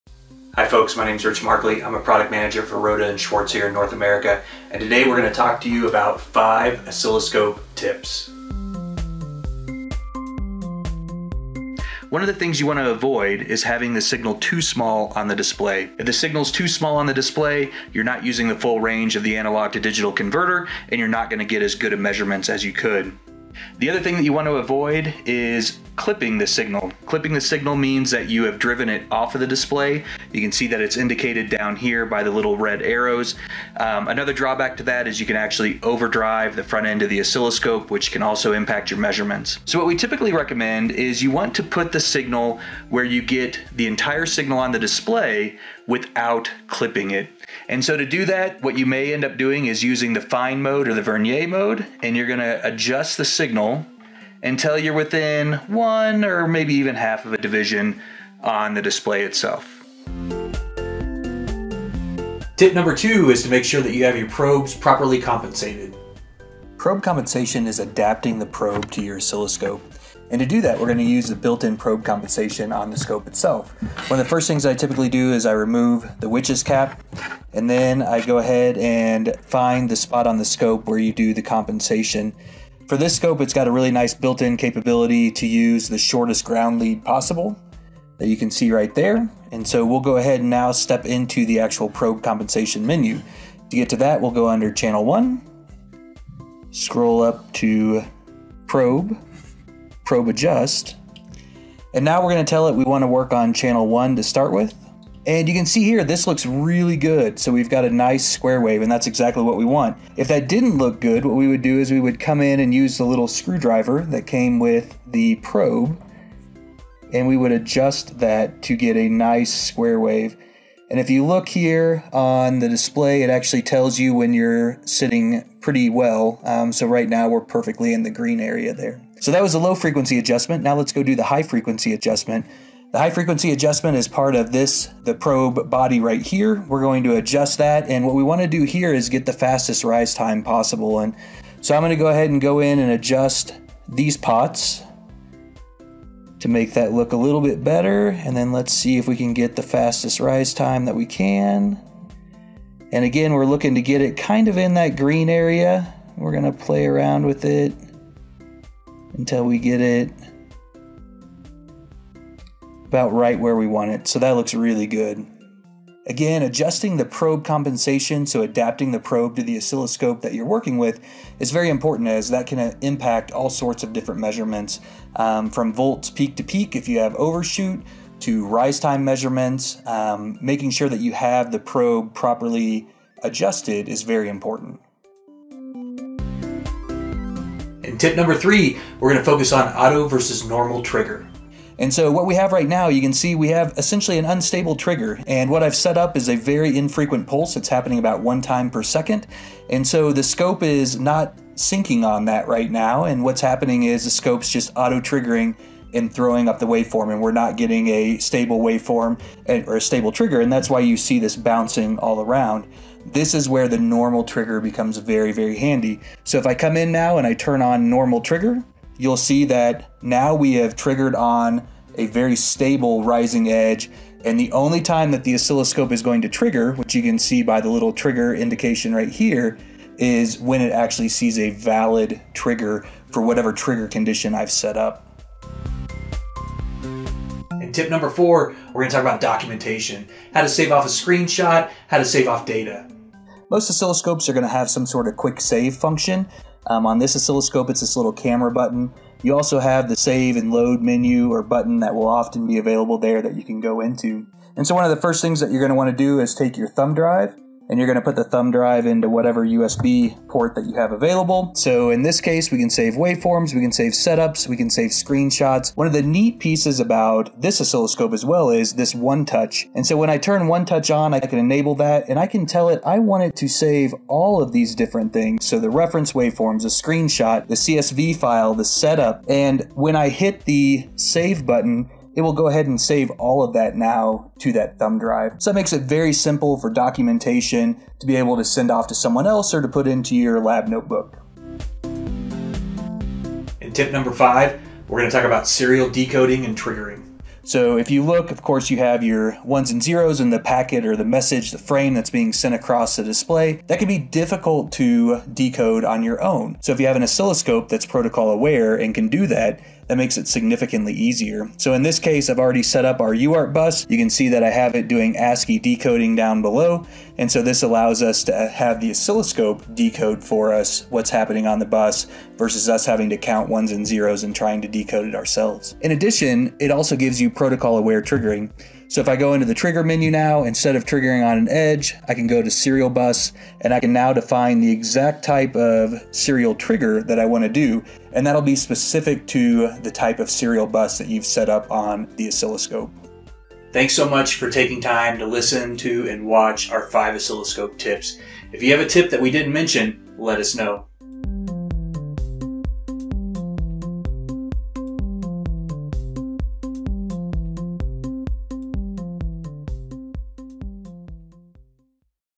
5 Oscilloscope Tips from the Rohde & Schwarz expert
Rohde & Schwarz Product Manager Rich Markley shares his top oscilloscope tips:
00:00 Start Video
00:11 Position the signal on the display: Avoid having the signal too small or too big
01:01 Properly compensate those passive probes!
03:06 Auto vs Normal Trigger
03:56 Documentation: Share your work. Save it for later.
04:53 Serial Decode and Triggering. Let the scope do the work for you!